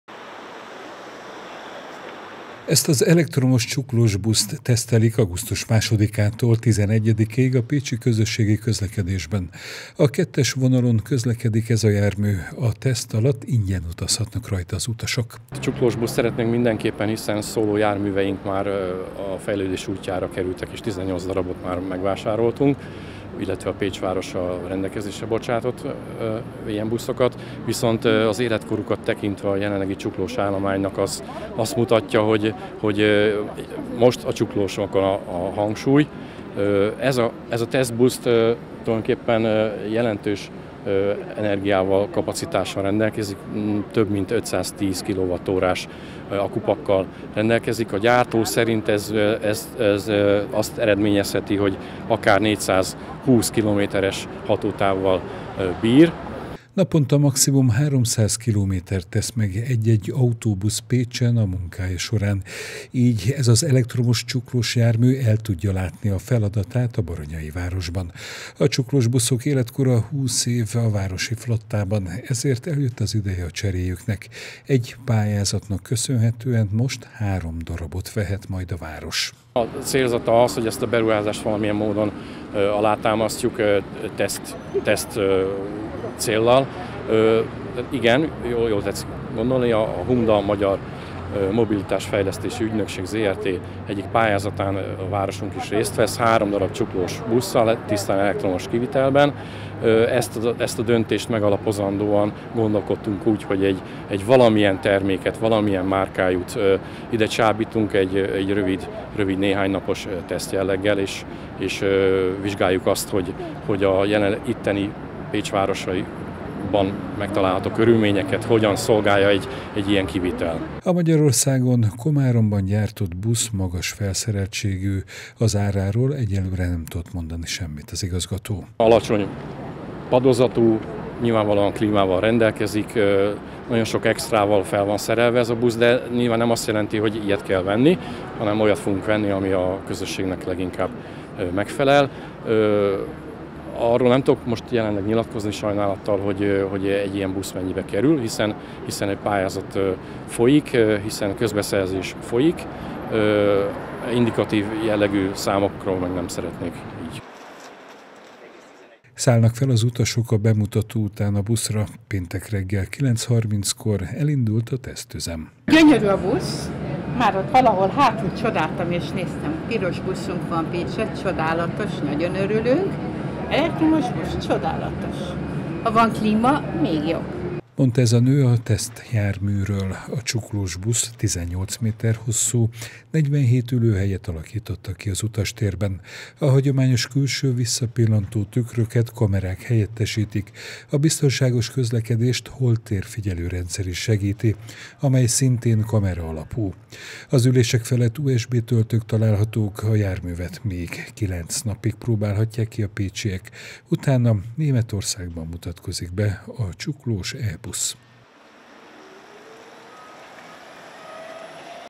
Három, elektromos csuklós buszt vehet Pécs - már tesztelik az egyik lehetséges jelöltet
Piros színű, teljesen elektromos meghajtású és csuklós kivitelű autóbusz jár a 2-es vonalon Pécsen, péntektől. A jármű tesztüzemre érkezett a Tüke Buszhoz. Egy pályázatnak köszönhetően három elektromos csuklós buszt vehet hamarosan a város. A próbajárat a megfelelő gyártmány kiválasztását segíti.